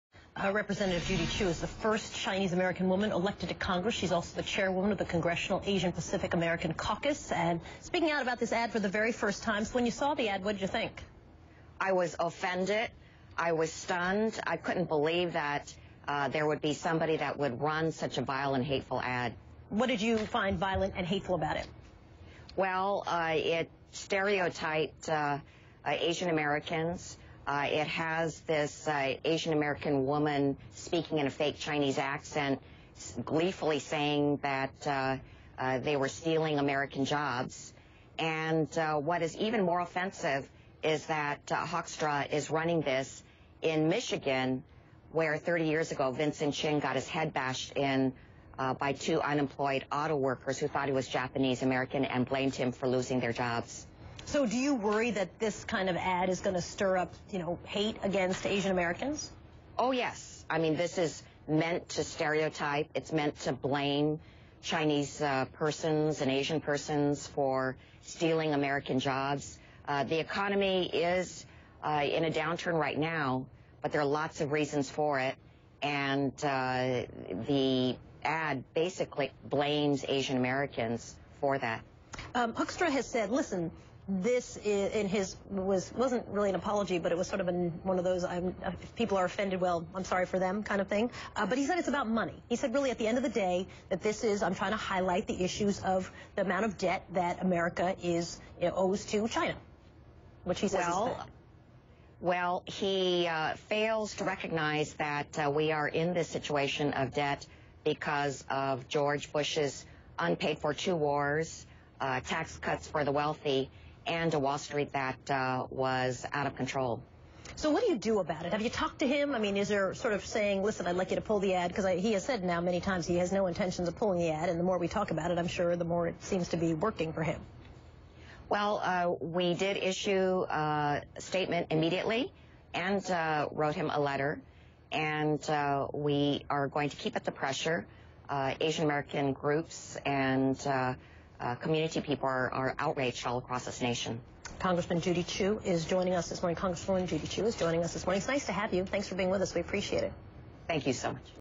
CAPAC Chair Rep. Judy Chu Speaks Out Against Hoekstra Ad
CAPAC Chair Rep. Judy Chu's CNN Interview with Soledad O'Brien  re Hoekstra Ad